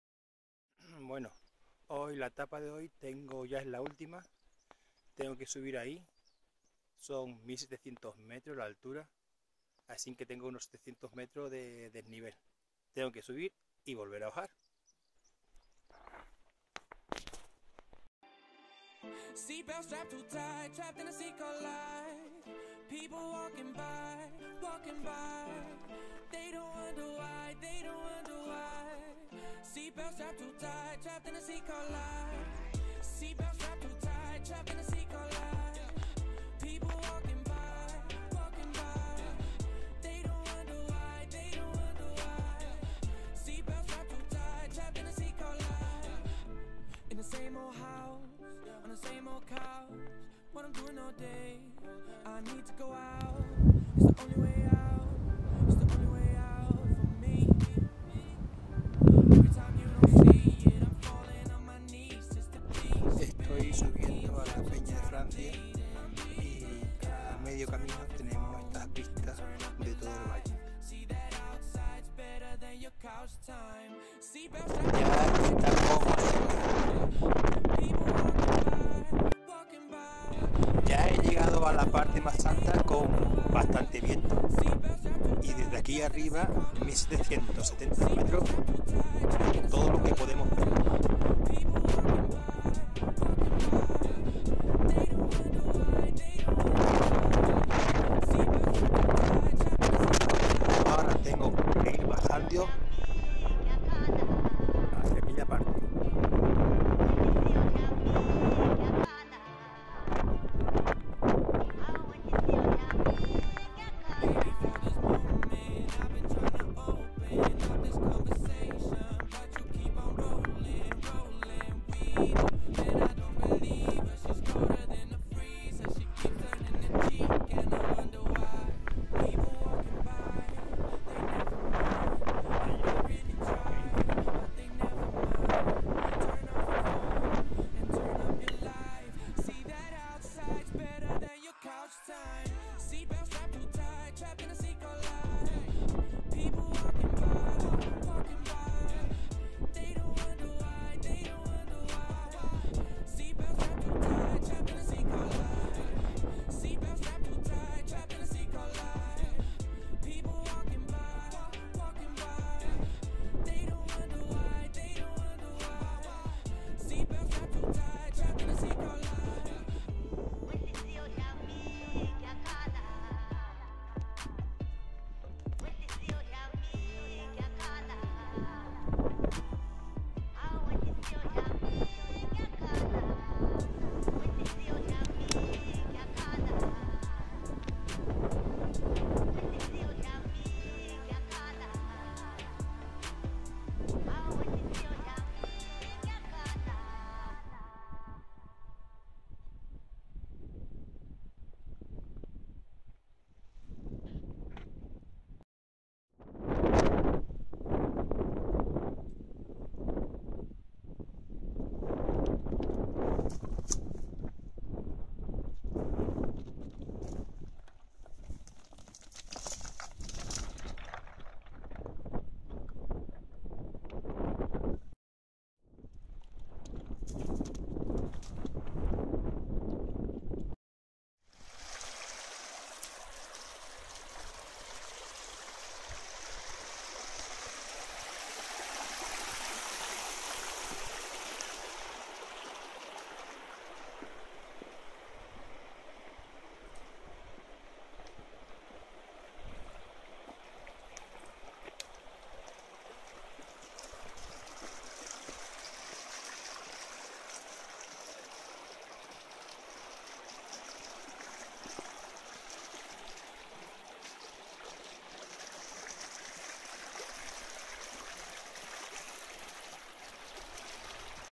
GR 10 Norte 6º día. La Alberca – Peña de Francia
Durante el mes de marzo del 2019, realice andando el GR 10 Norte, es la parte que pasa por la provincias de Salamanca. Fueron seis etapas desde Bejar hasta La Alberca. Para ver más mirar en: